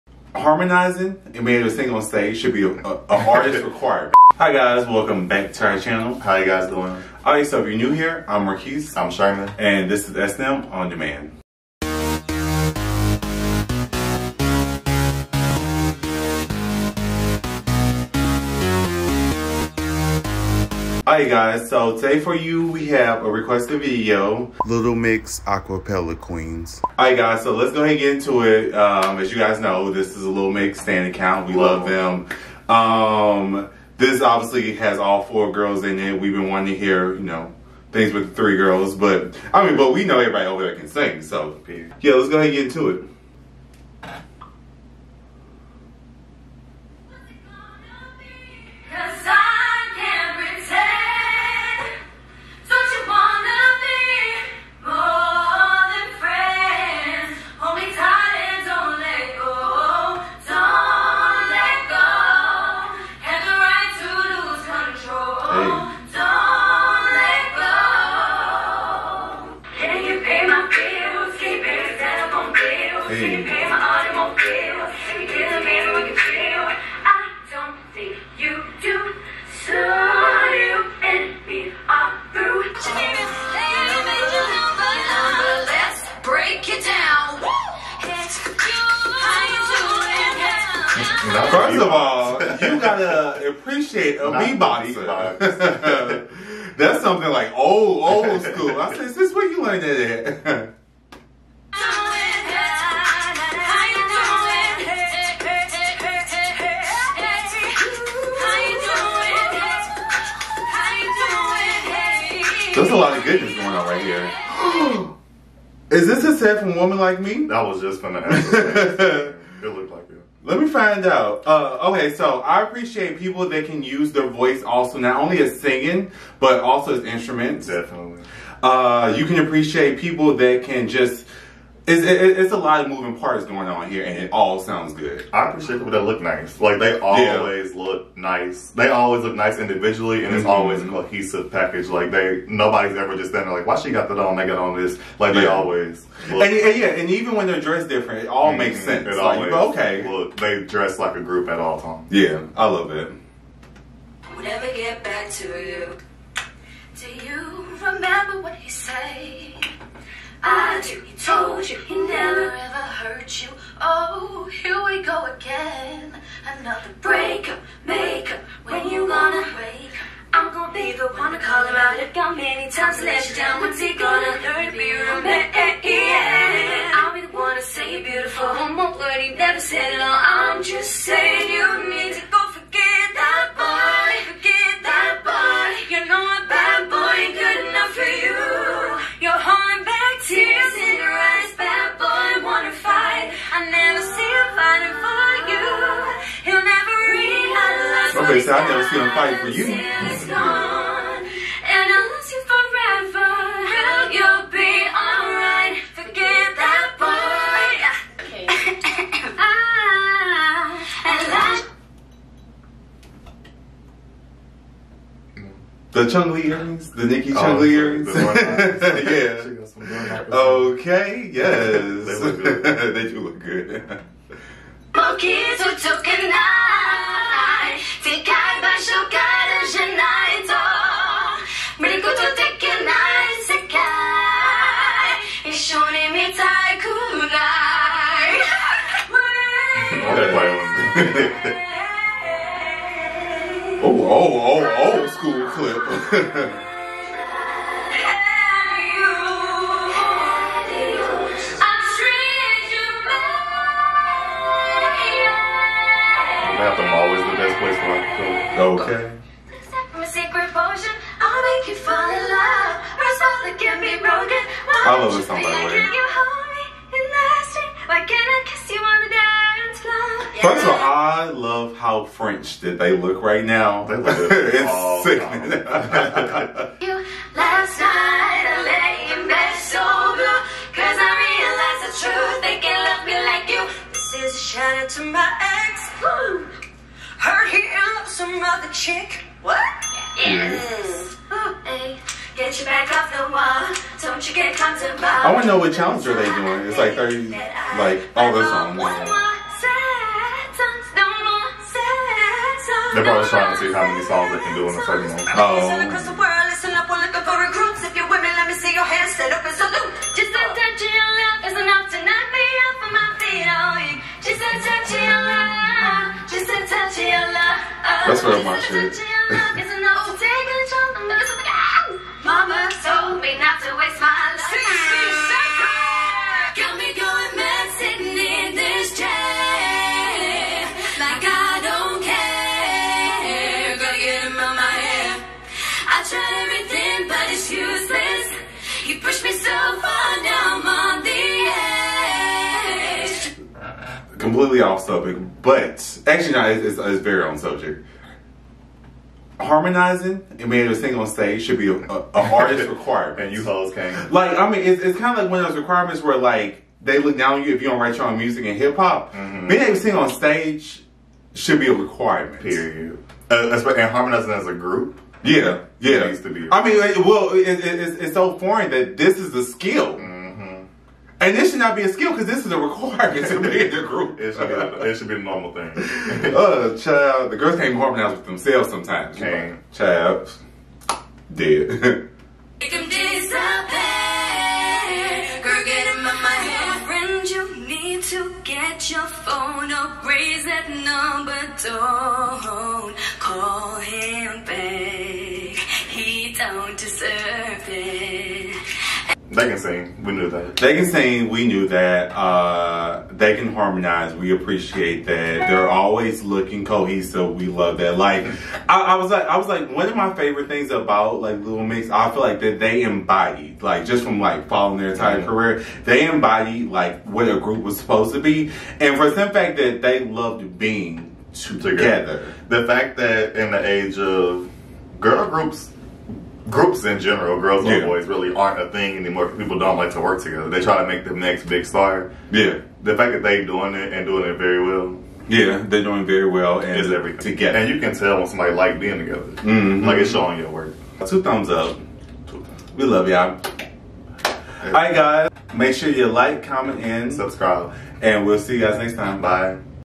These Girls Can Sang - Little Mix Acapella Queens Reaction - We're Shook
We're shook but not surprised. Little Mix are the acapella queens at singing. Here's our reaction.